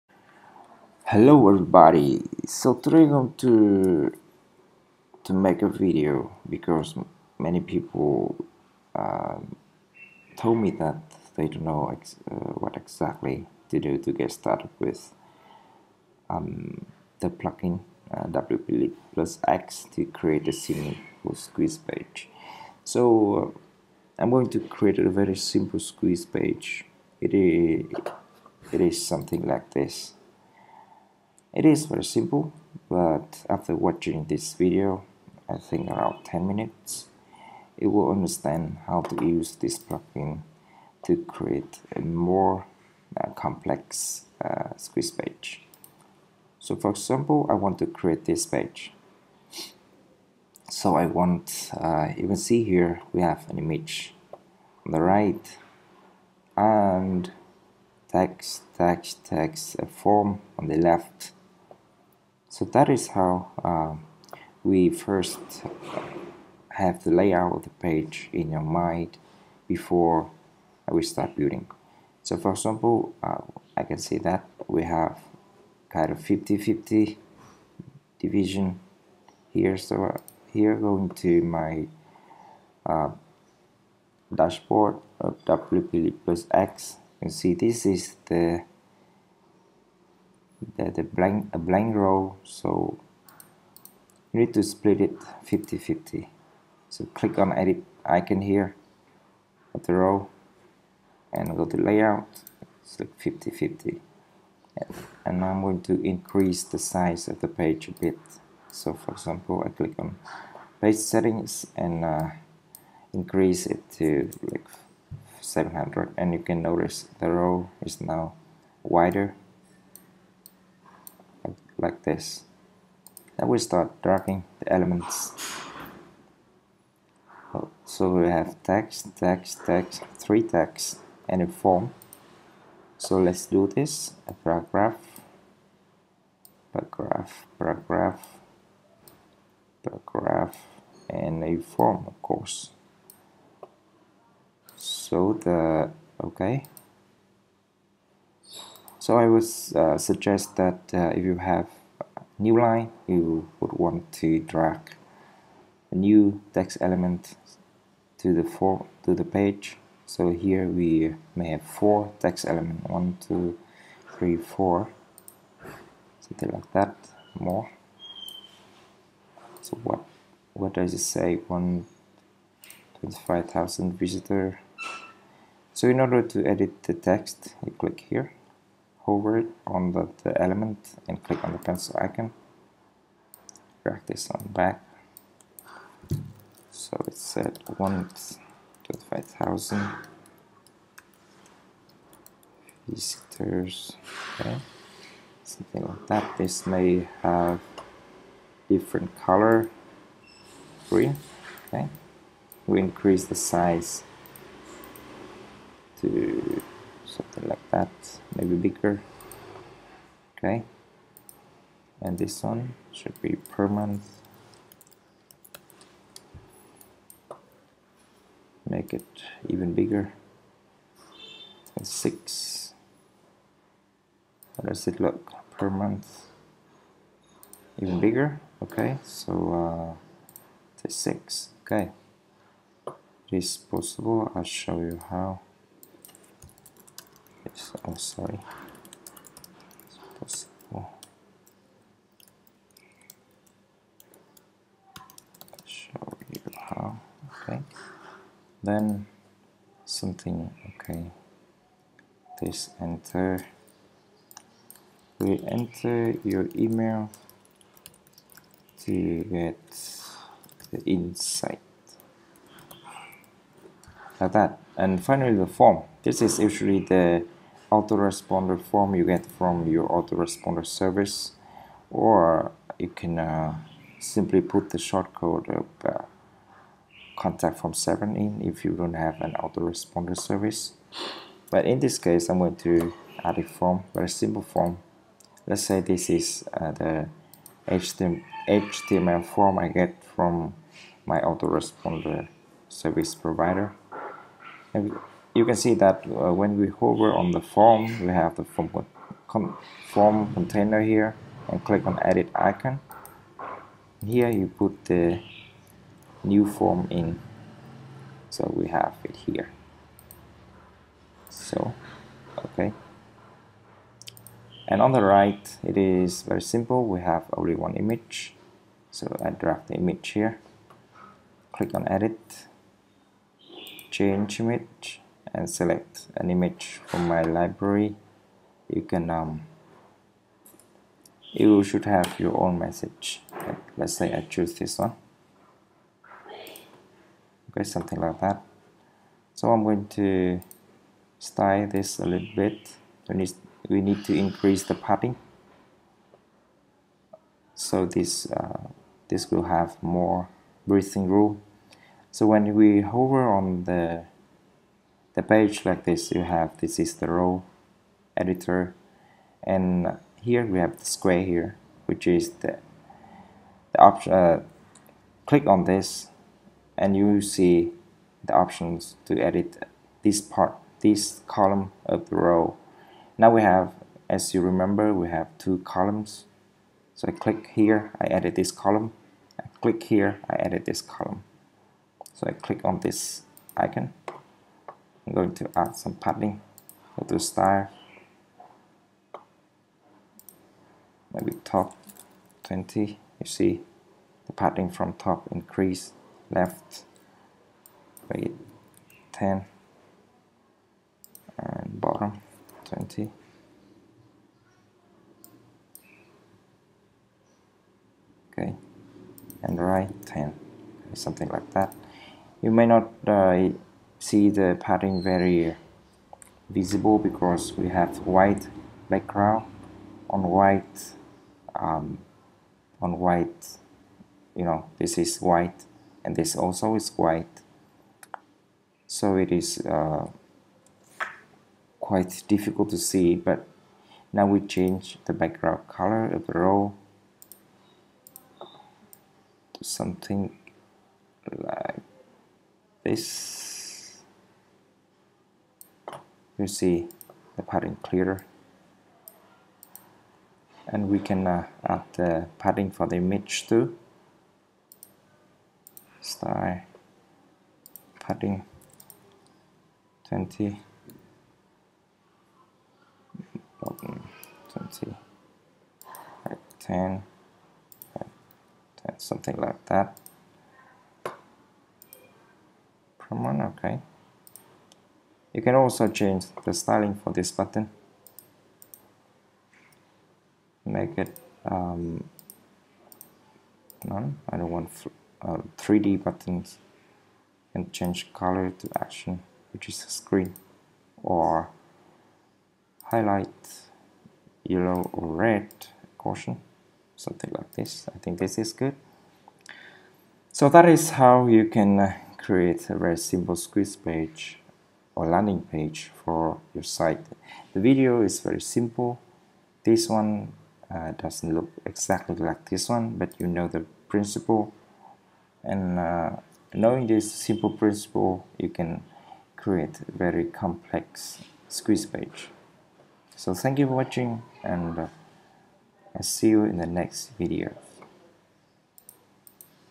How To Create A Simple Squeeze Page, Landing Page In WordPress Using WP Lead Plus X
Using WP Lead Plus X to create landing pages, squeeze pages.